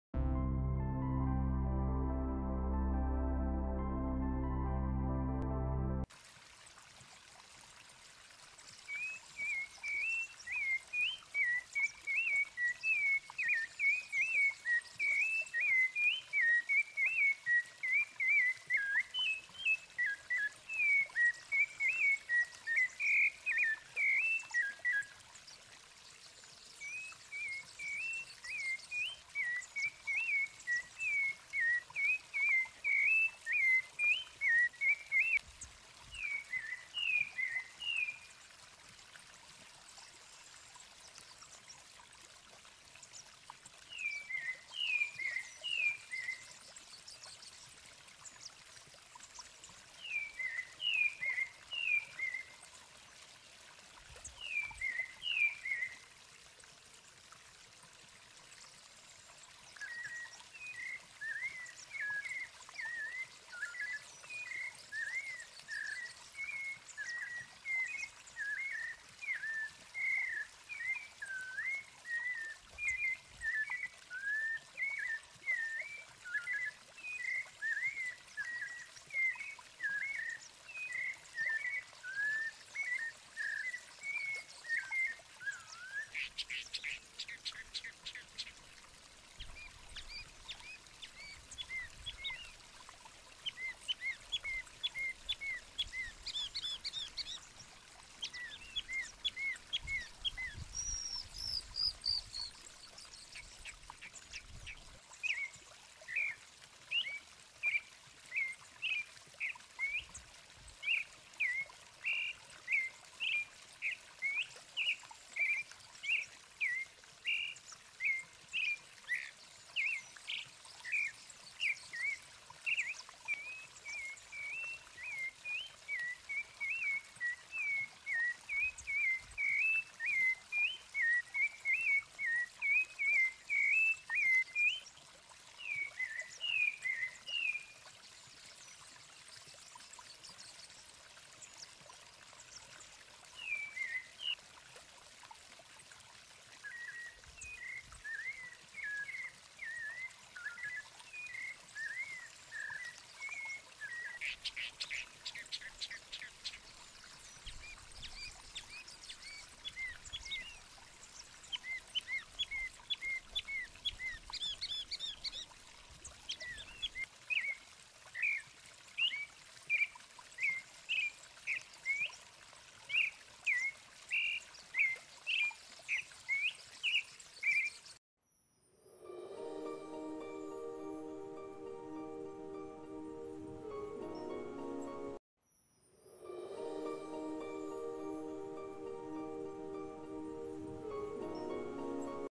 SABIÁS.Conheça 5 belos sabiás do Brasil e seus cantos.
Animal * Conheça 5  belos sabiás do Brasil e seus cantos.